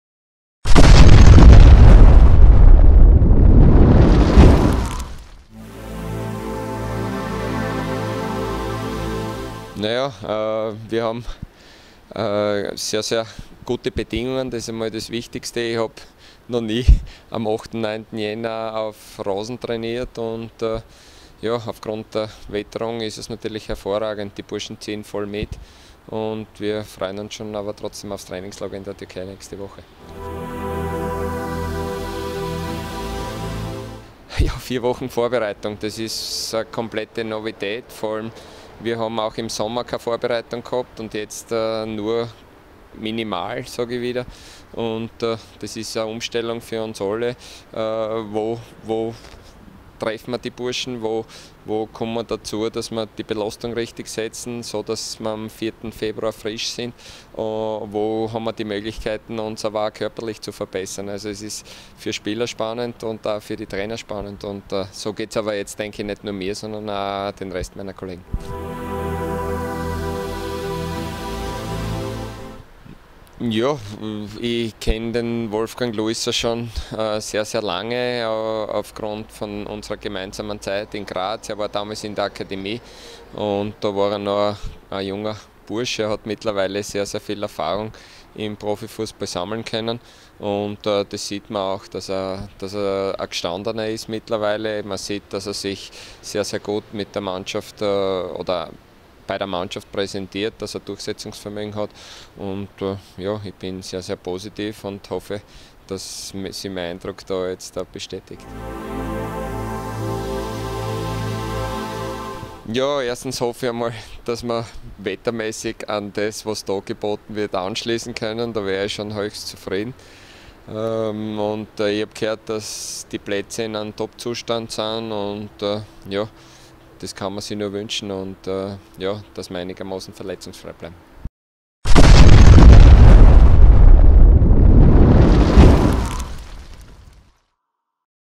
SCRA TV 2018 Wintervorbereitung - Klaus Schmidt
Der Trainer ist mit den bisherigen Trainingsbedingungen sehr zufrieden und hofft auch im Trainingslager die nächsten Schritte mit der Mannschaft gehen zu können.